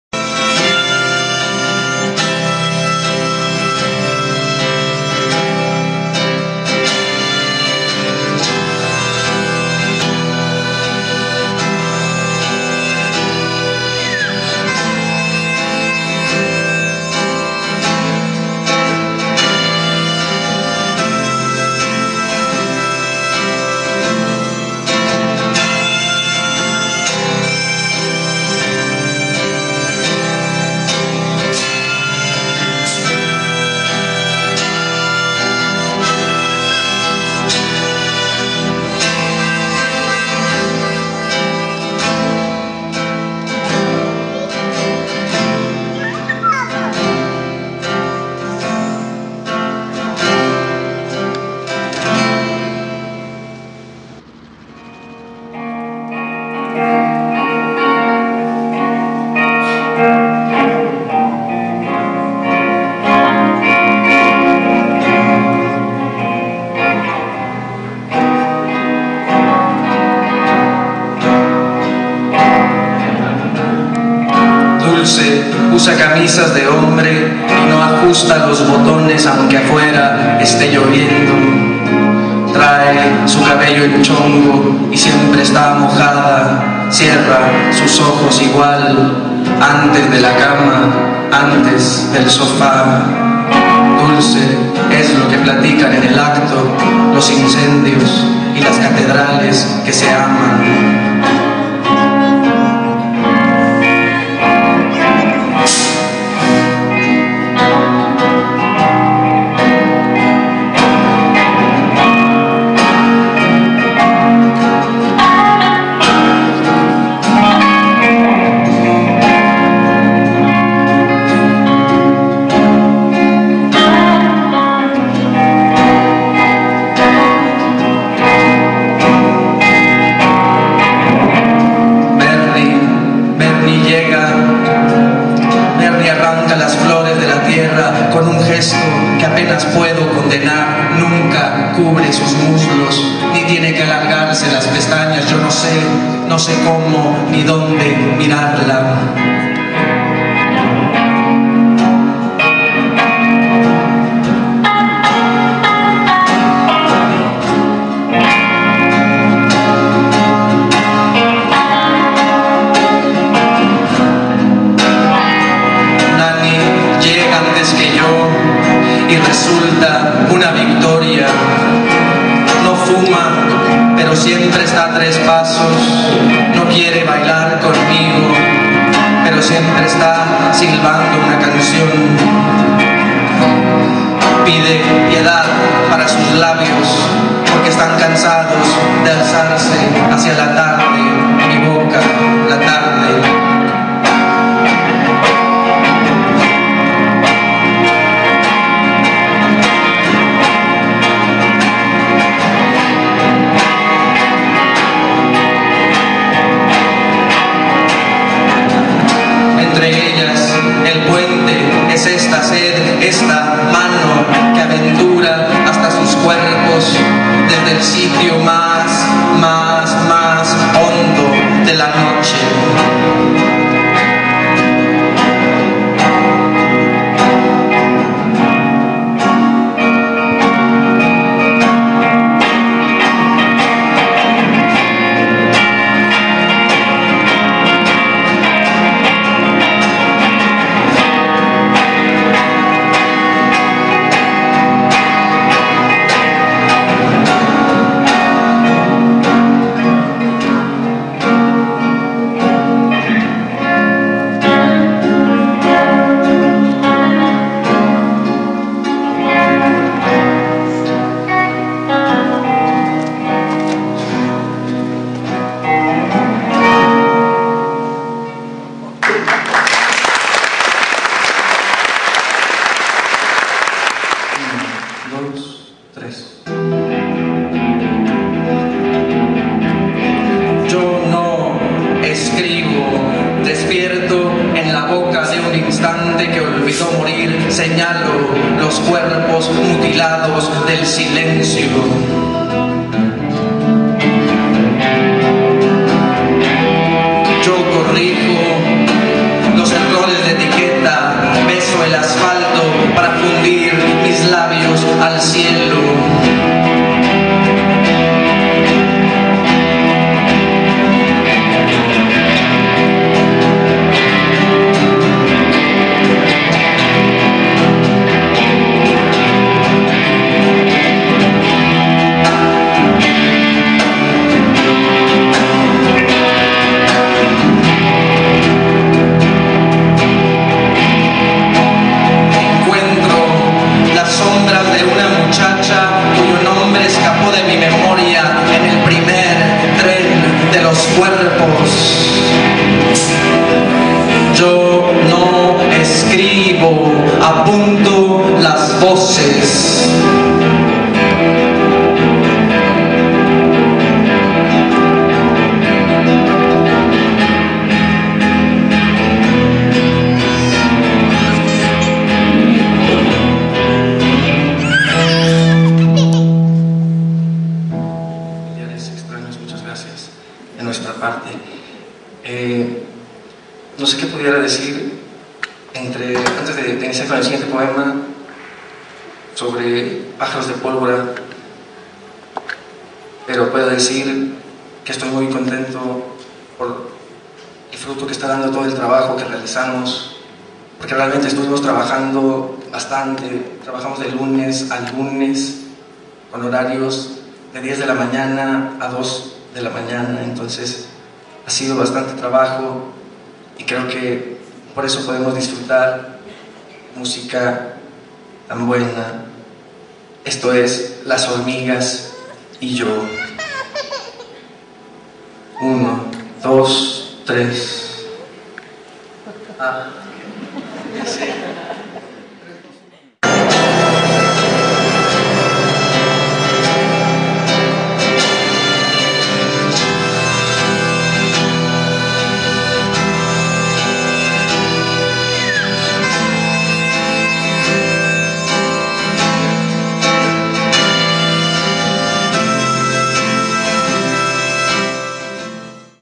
José Chapa " En El Teatro de Cámara" del Teatro de la Ciudad 3 de 4
José Chapa Inauguró el Teatro de Càmara del Teatro de la Ciudad de Monclova.
Foto y Video. Ernesto Sifuentes